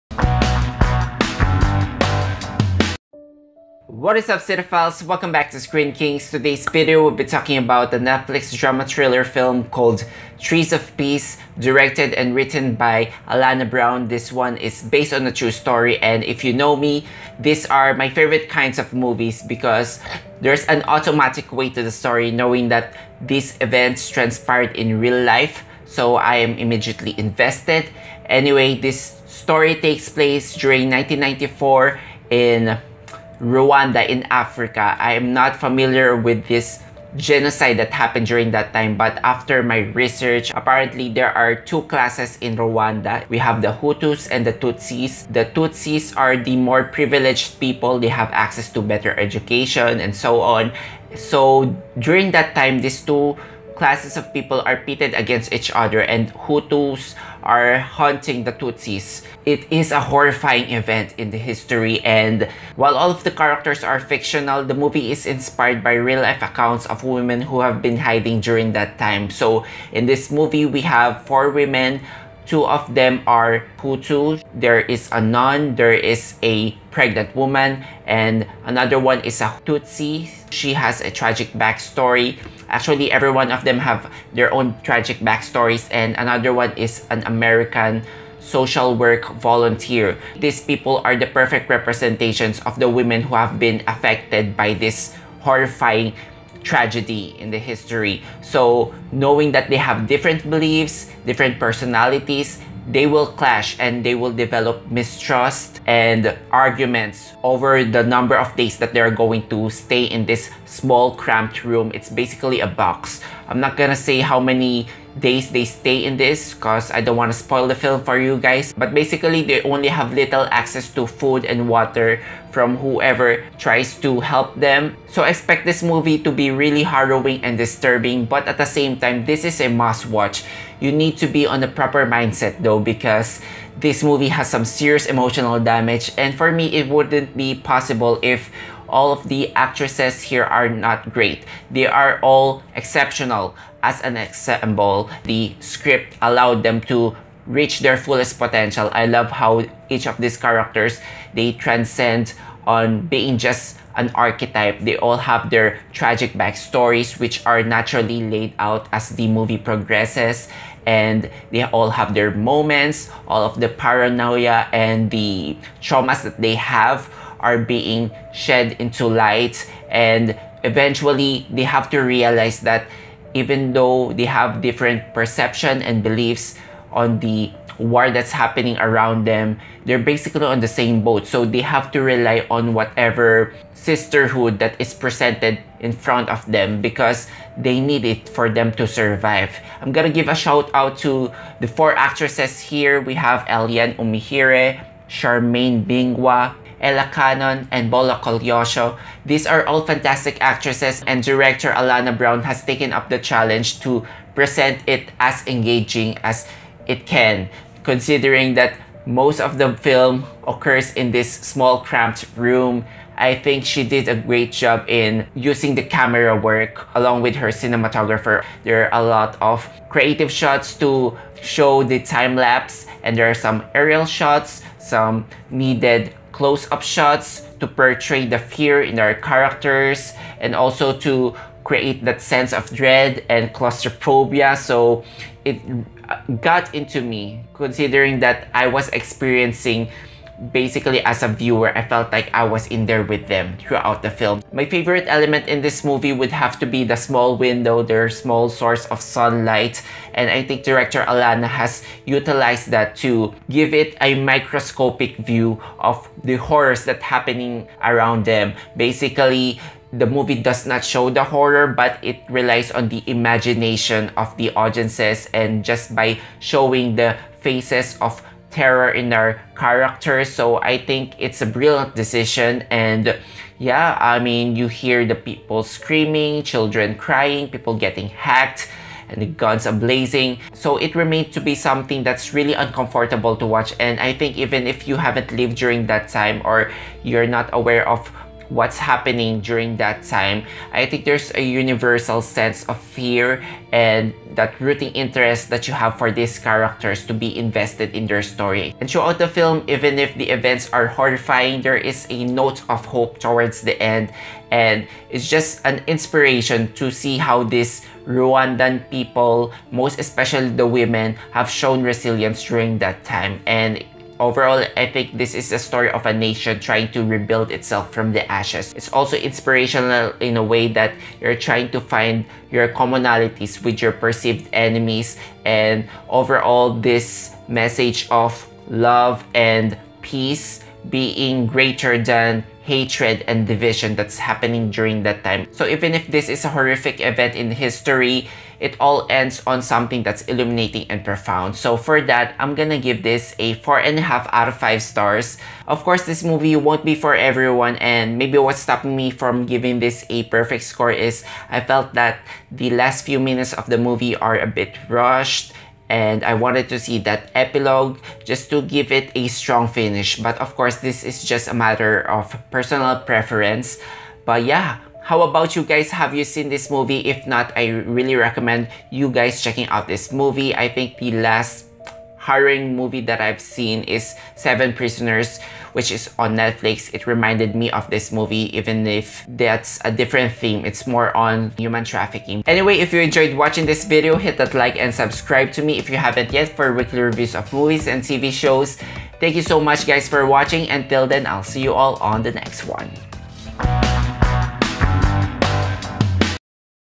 Trees of Peace Netflix review | Rwanda genocide movie
In April 1994, four women from different backgrounds and beliefs are trapped and hiding during the Genocide Against the Tutsi in Rwanda. Here's my review of Netflix's latest hidden gem inspired by true events, TREES OF PEACE.

Directed and written by Alanna Brown, 'TREES OF PEACE' stars Eliane Umuhire, Charmaine Bingwa, Ella Cannon, Bola Koleosho and Tongayi Chirisa. Distributed by Netflix.

Let us know what you think! Hit the comment section below!